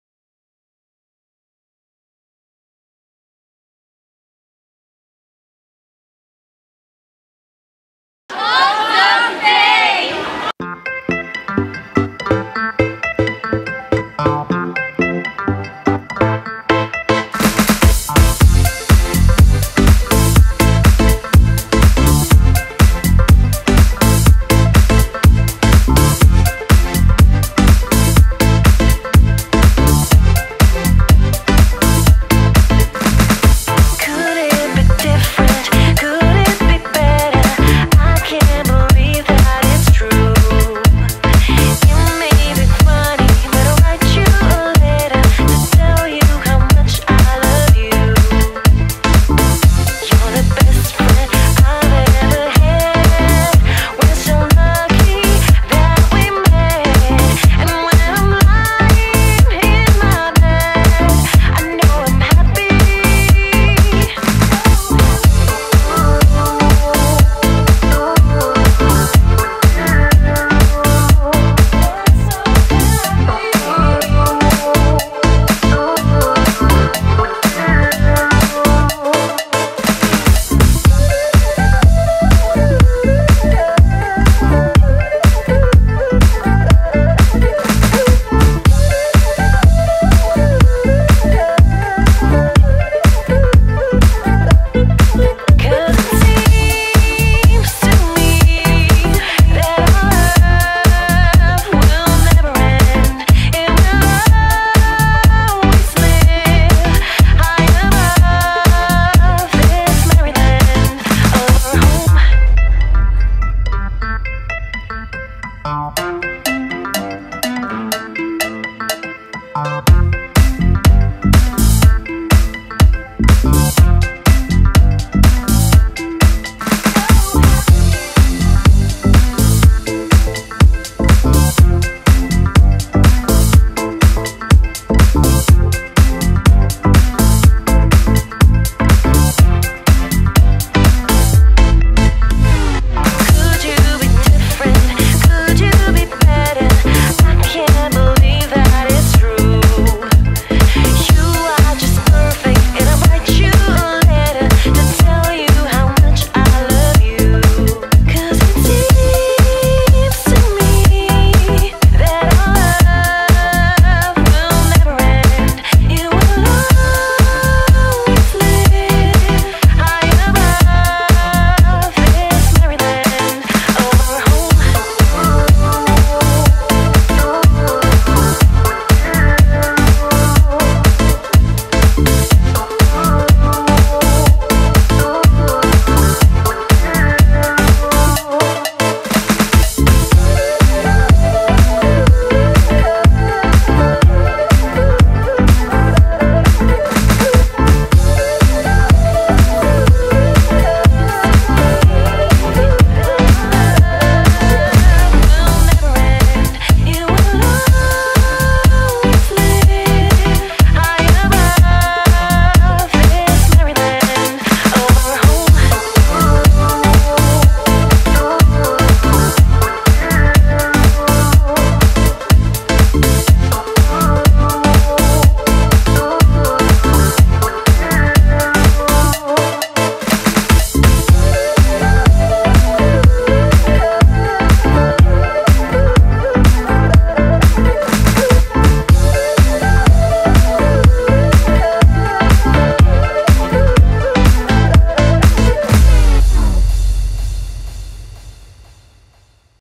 Odd Socks Day_British School of Valencia
¡Ayer todos celebramos OddSocksDay🧦 y lo pasamos en grande! Luchemos todos juntos contra el acoso escolar, ChooseRespect.

Yesterday we all celebrated OddSocksDay 🧦 and we had a great time! Let’s take a stand against bullying, ChooseRespect.